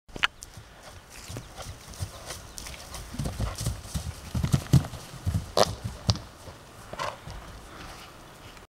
Jake misbehaving
first time out of doors, all a little much for Jake!